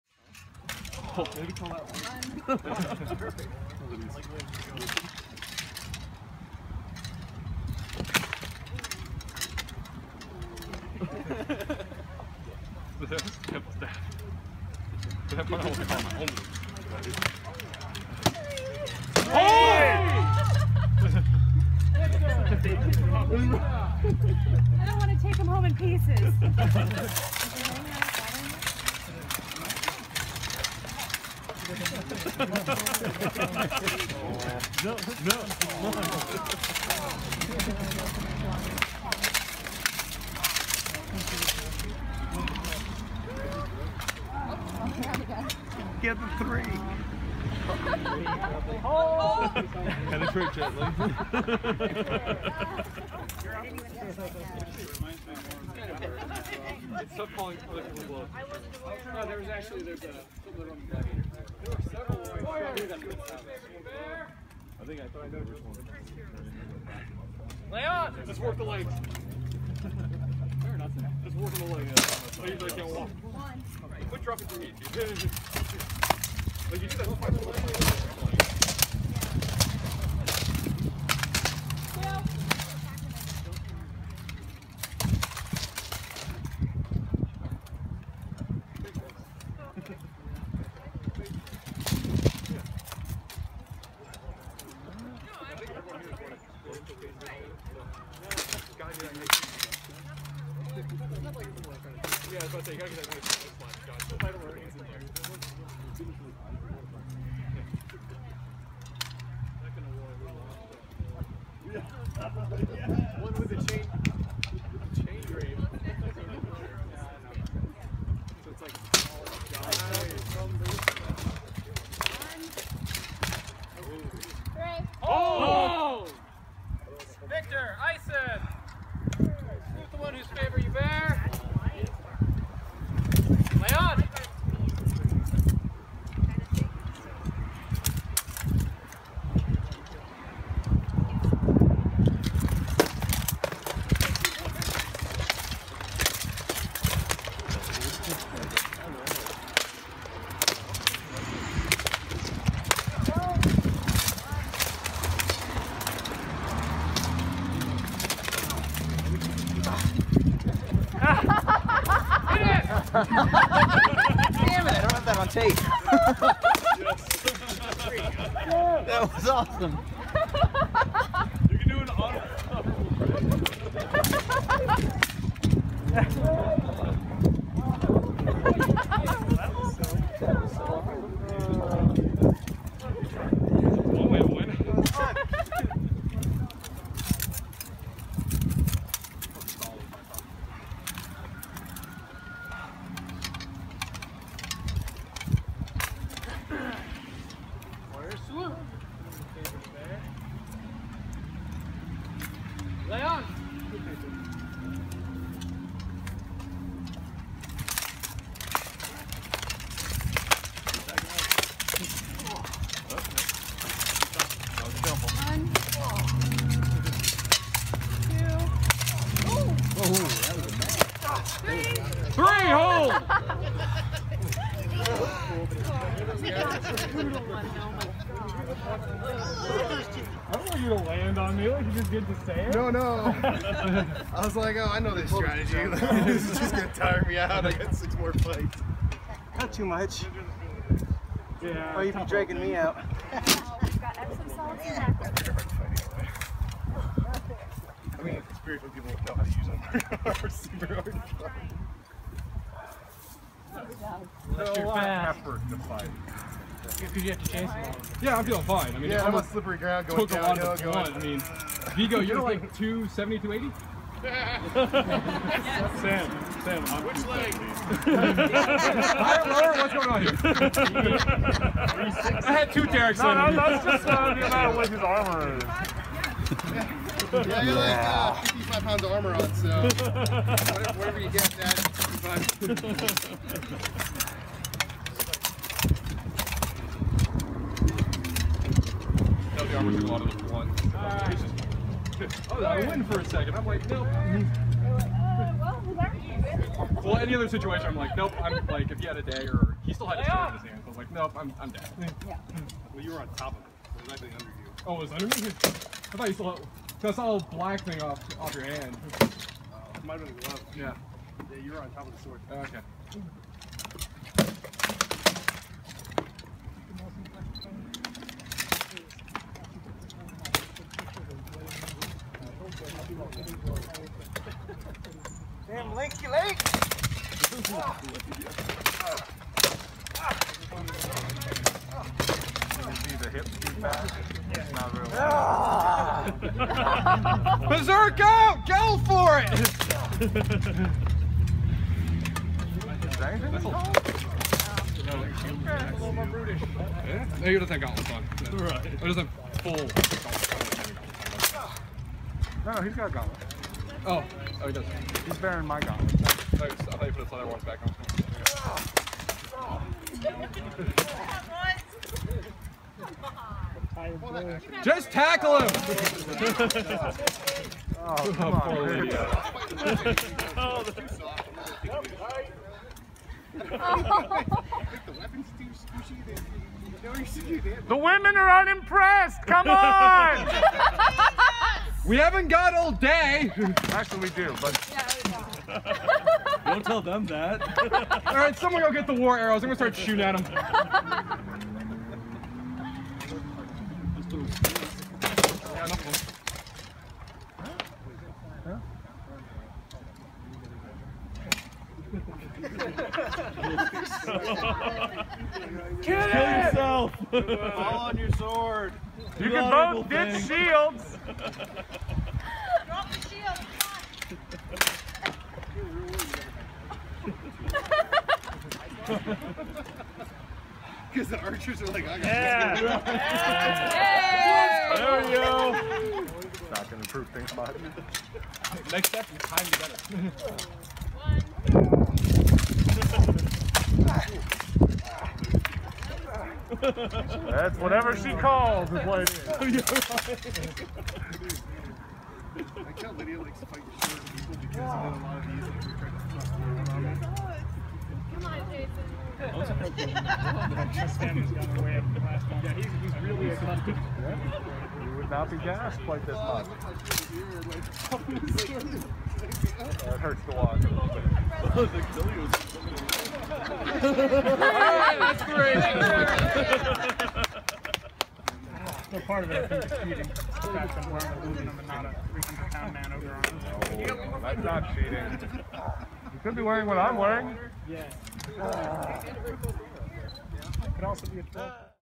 2016 Feast of Saint Valentine's Tournament of Favor
Iason Grønn-Drengr fights for the Hrafn Halda branch of the Kingdom of Ardesca.